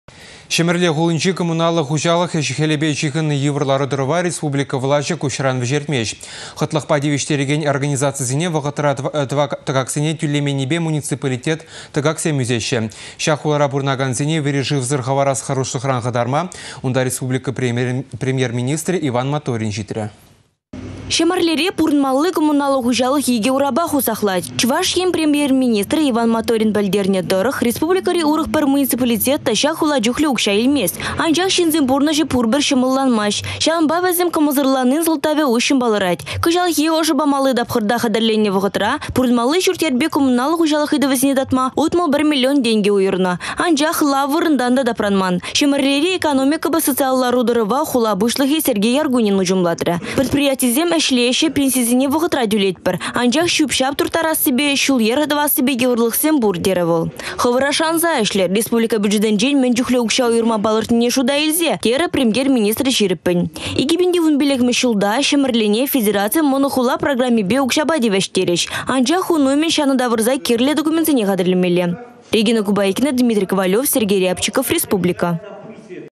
Çĕмĕрле парăмĕсем патшалăх бюджетне «çĕмĕреççĕ»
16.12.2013
Çĕмĕрле хулинчи комуналлă хуçалăх ĕç-хĕлĕпе çыхăннă йывăр лару-тăрăва республика влаçĕ куçран вĕçертмест. Хăтлăхпа тивĕçтерекен организацисене вăхăтра тăкаксене тÿлеменнипе  муниципалитет тăкакĕсем ÿсеççĕ. Çак хулара пурăнакансене вĕри шывсăр хăварас хăрушлăхран хăтарма унта республика премер-министрĕ Иван Моторин çитрĕ.